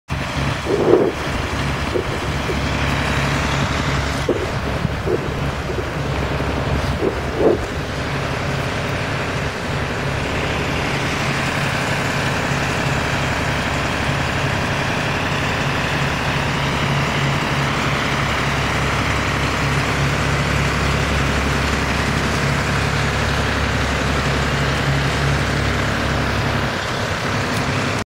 Lochgelly alpha 41 at secen of localised flooding in Ballingry
Lochgelly alpha 41 at secen of localised flooding in Kirktoun park in Ballingry